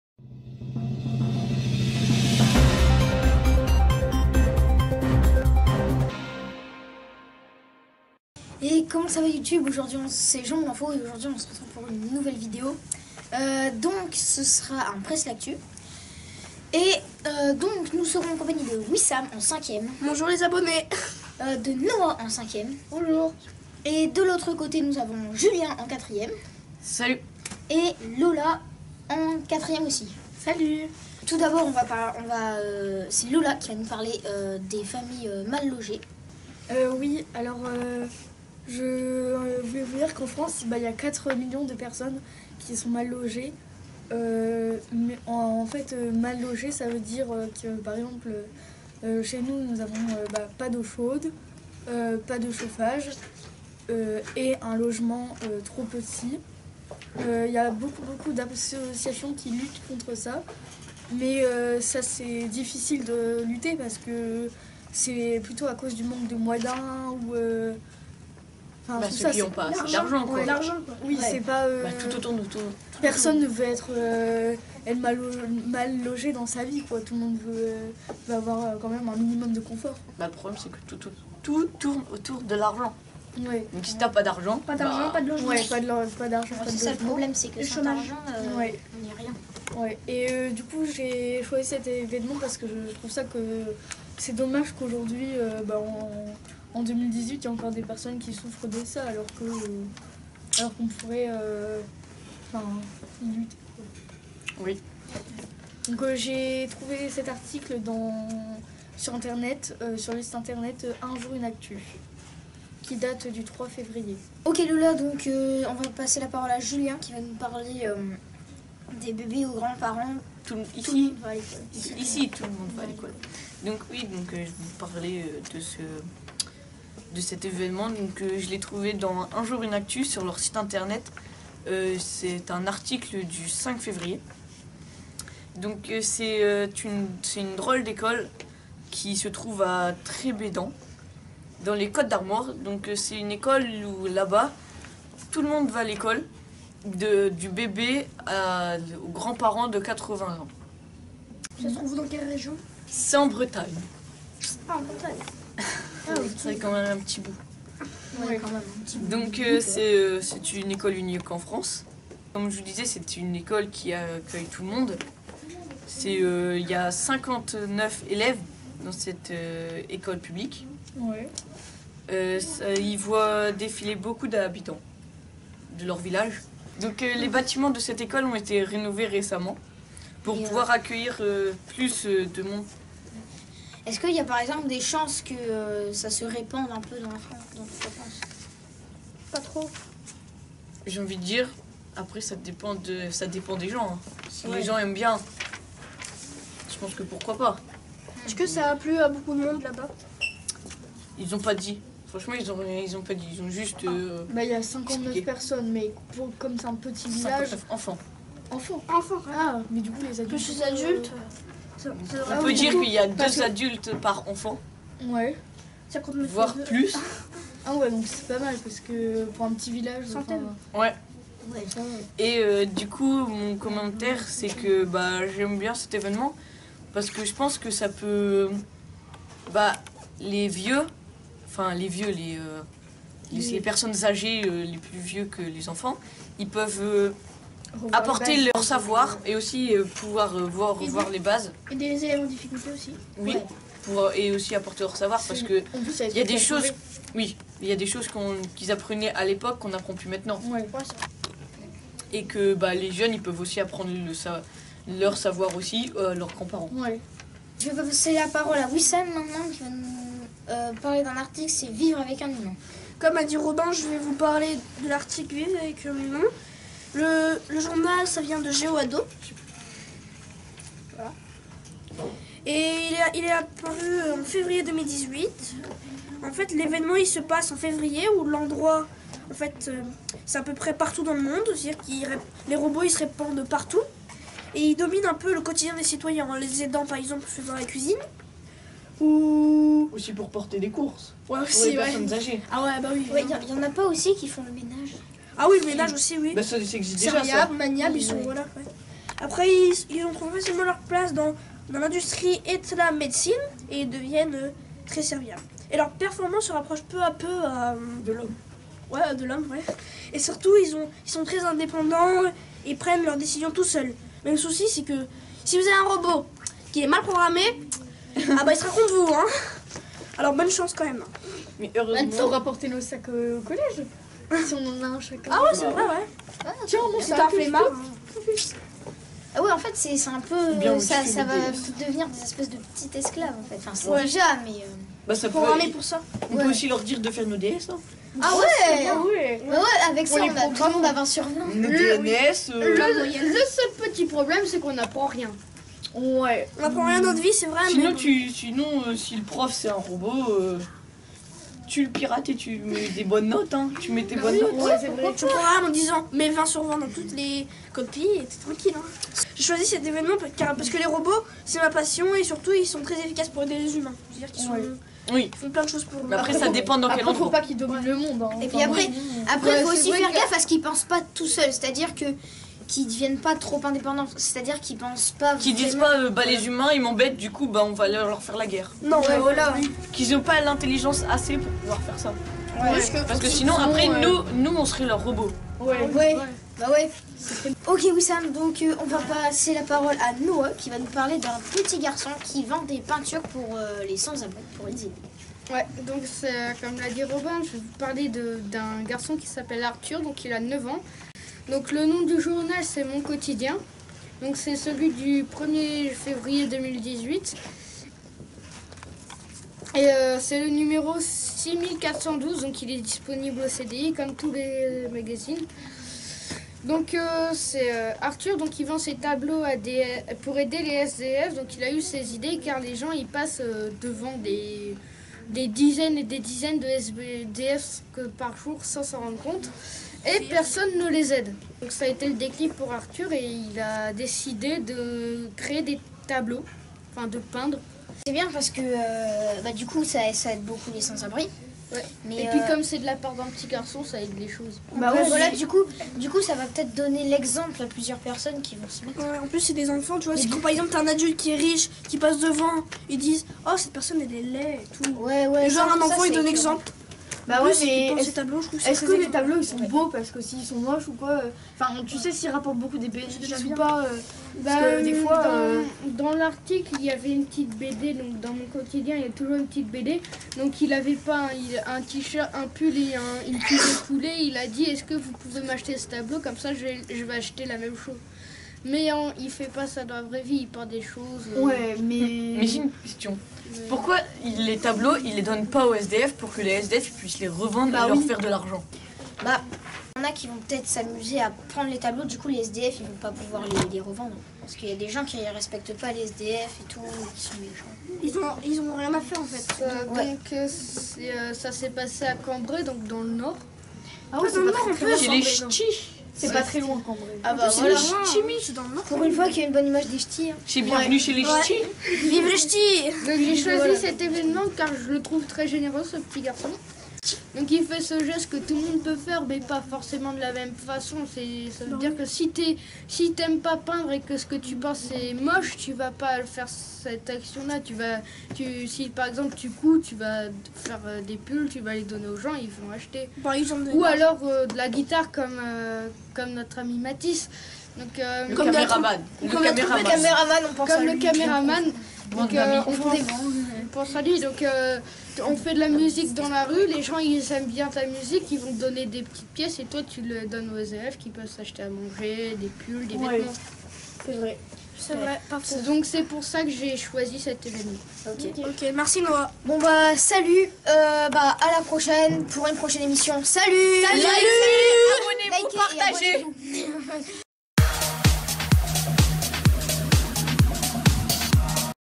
PRESSE L'ACTU n°19 - Fév 2018
Présentée par Robin, de 5 ème, cette émission passe en revue l'actualité de février 2018. Avec Noa et Wissam de 5 ème et Julien et Lola de 4 ème. Ils sont filmés par Mathis, de 5 ème.